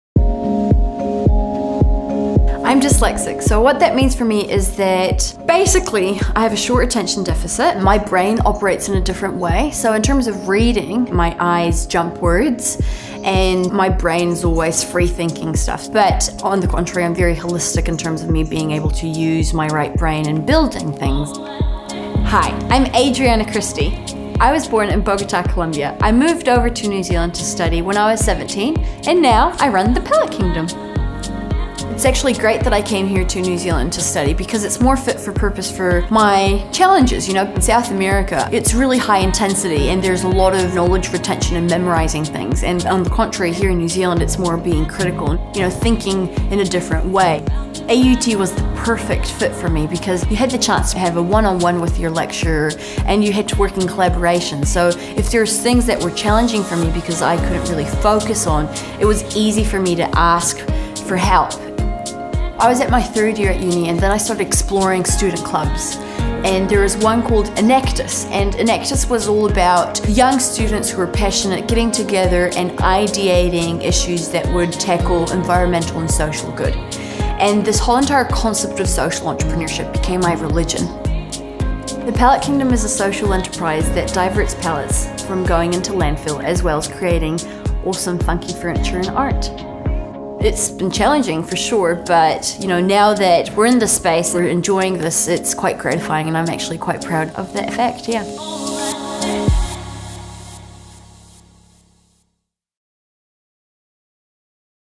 NZ Alumni- How to become a successful business owner.. (Bachelor of Business- Honours)
Adriana is from Bogotá,Colombia. She is one of our awesome NZ Alumni🎓 and is now the founder of The Pallet Kingdom👑 in Auckland. How to Apply to Study in New Zealand

She studied a Bachelor of Business Studies (Honours) at AUT - Auckland University of Technology.
What makes her story more inspiring is that she is dyslexic but, was able to overcome this and become a successful business owner at the end🙌. Find out more about her inspiring StudyinNZ journey here⬇️.